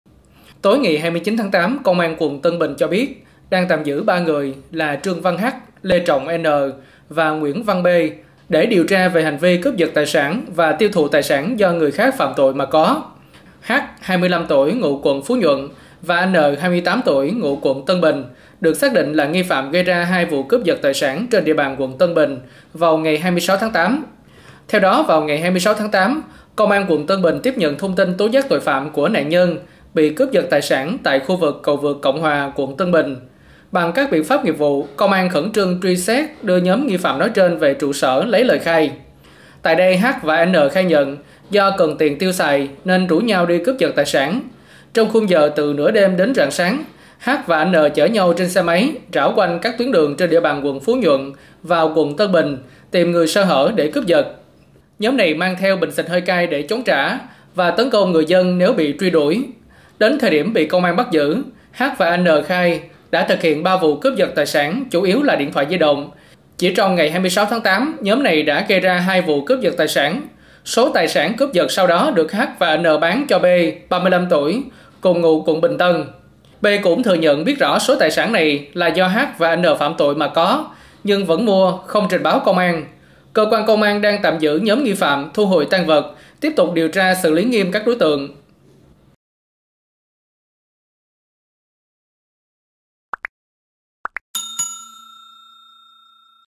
Công an bắt giữ nhóm cướp giật tài sản mang theo bình xịt hơi cay
Tối 29/8, Công an quận Tân Bình cho biết đang tạm giữ 3 người là Trương Văn H, Lê Trọng N và Nguyễn Văn P để điều tra về hành vi cướp giật tài sản và tiêu thụ tài sản do người người khác phạm tội mà có.